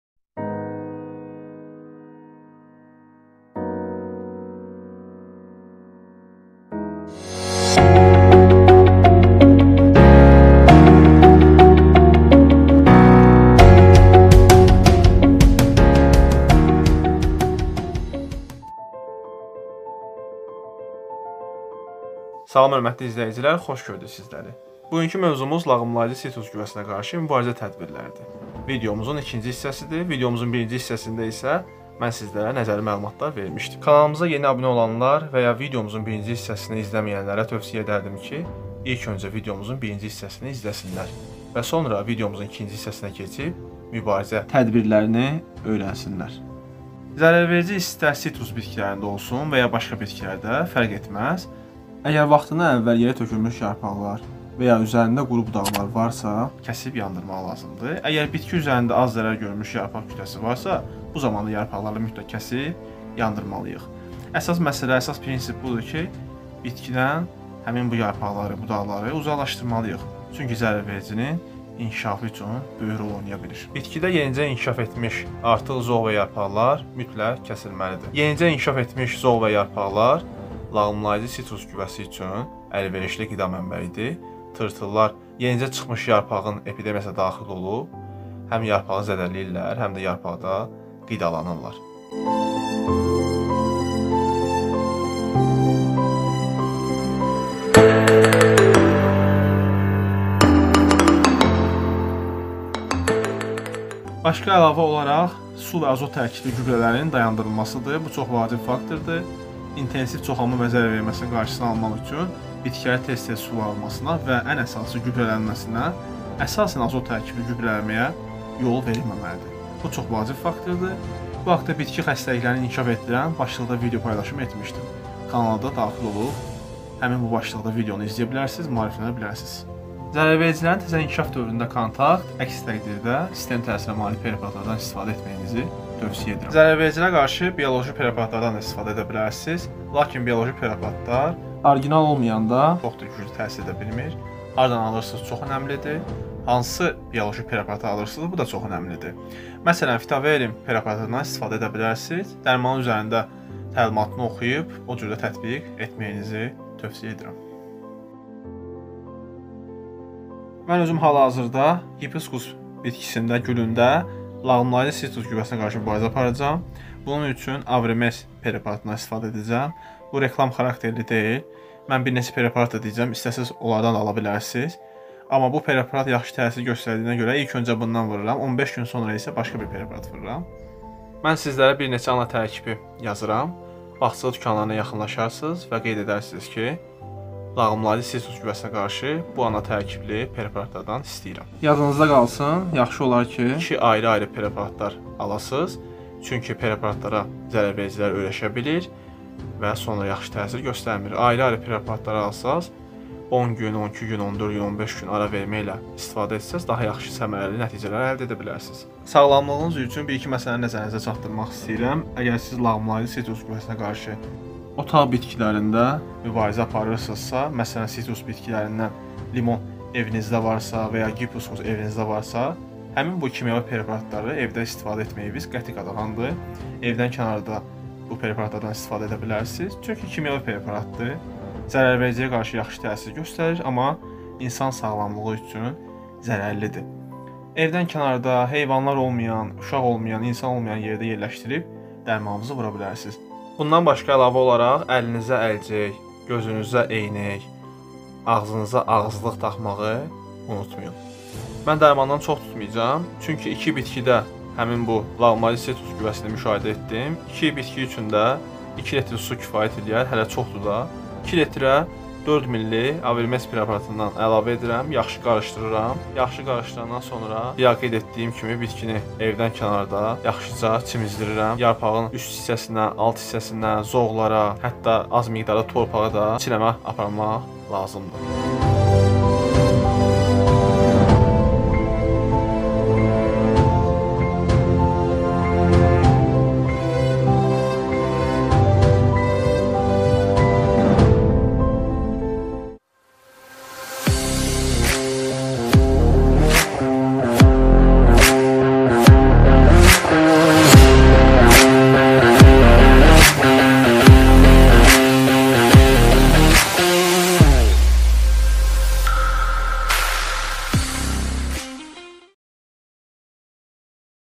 🐛🌳 Limon agacına qulluq | limon ziyanvericisi - SİTRUS GÜVƏSİ - YAPRAK GALERİ GÜVESİ / 2-Cİ HİSSƏ
📋Bizimlə əməkdaşlıq üçün,
aşağıdakı whatsApp nömrəsi ilə əlaqə saxlaya bilərsiz:

" LAĞIMLAYICI SİTRUS GÜVƏSİ  ".